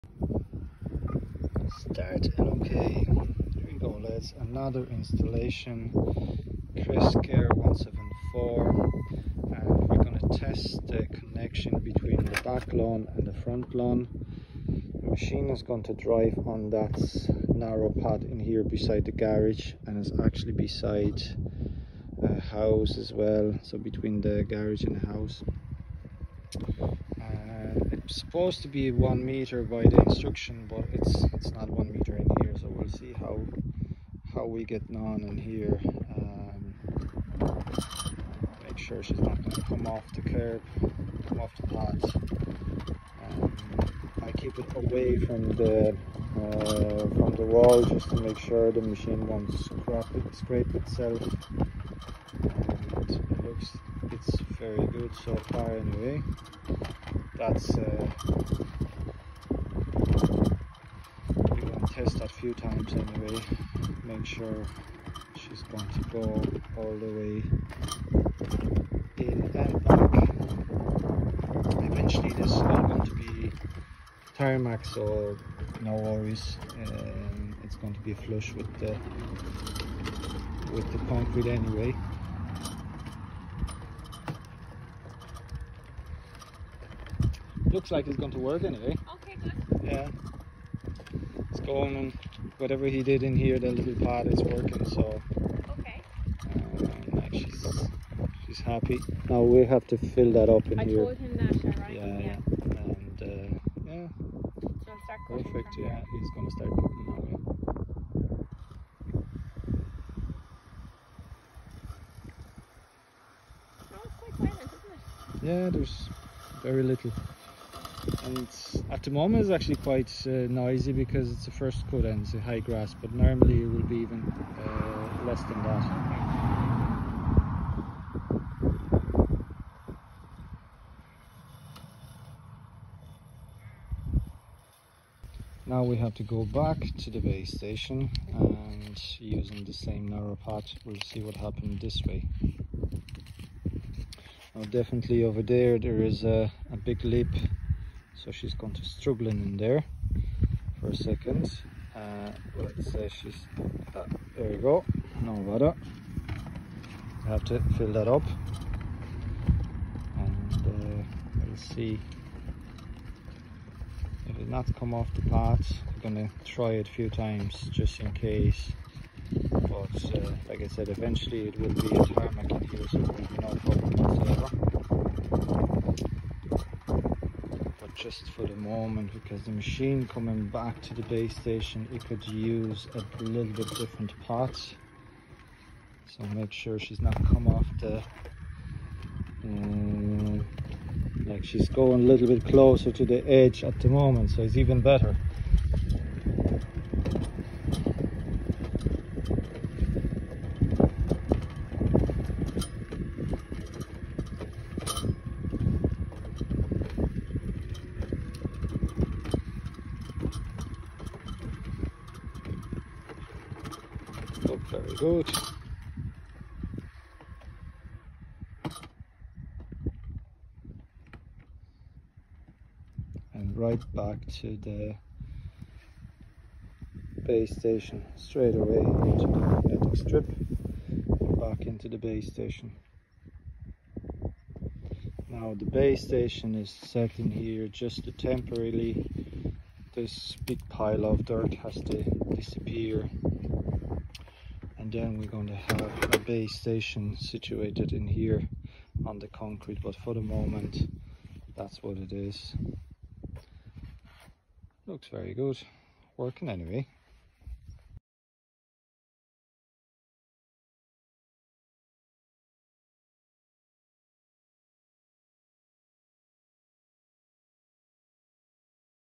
Kress KR174E
In this video, we present the Kress KR174 robotic lawn mower, which stands out with its precise mowing capabilities and modern GPS technology. The mower operates without the need for boundary wires, making it ideal for large and complex gardens.

Technical Specifications:

 • RTKn Navigation System: The mower uses an advanced RTK system that provides centimeter-level accuracy without the need for antenna installations on the property .
 • Inertial Navigation: When satellite signals are weak or obstructed by buildings or trees, the mower switches to inertial navigation, maintaining accurate operation .
 • OAS (Obstacle Avoidance System): The obstacle avoidance system allows the mower to efficiently navigate around obstacles on the lawn, increasing its efficiency and safety .
 • No Boundary Wires: Thanks to the RTKn technology, there is no need to install boundary wires, significantly simplifying the setup and use of the device .
 • Automatic Cutting Height Adjustment: The electronic cutting height adjustment allows easy customization of settings to meet user needs, ensuring perfect mowing every time .
 • Eco-Friendly Operation: The Kress KR174 is quiet and emission-free, making it environmentally friendly and suitable for use at any time of day or night .

In the video, you will see the mower leaving its base and following a narrow path next to the garage to another lawn area. With its precise navigation system and advanced technology, the Kress KR174 ensures a perfectly mowed lawn without the need for constant user intervention.

Conclusion

The Kress KR174 lawn mower is the perfect solution for those who value precision, convenience, and eco-friendliness. If you are looking for an innovative and effective robotic lawn mower, the KR174 will certainly meet your expectations.

For more information about the mower, visit the manufacturer’s website and check out our detailed review.